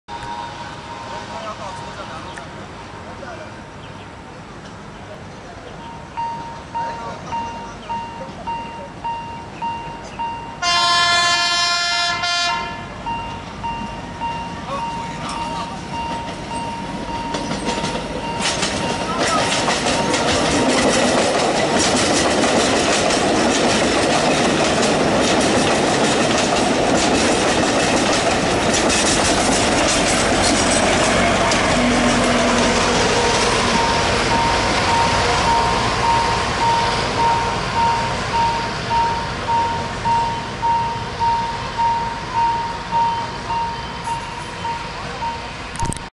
Alishan Forest Railway Train and Old lady with a death wish
DL 47 pushing empty passenger train return from Chiayi Station back to yard and little close call with an old lady.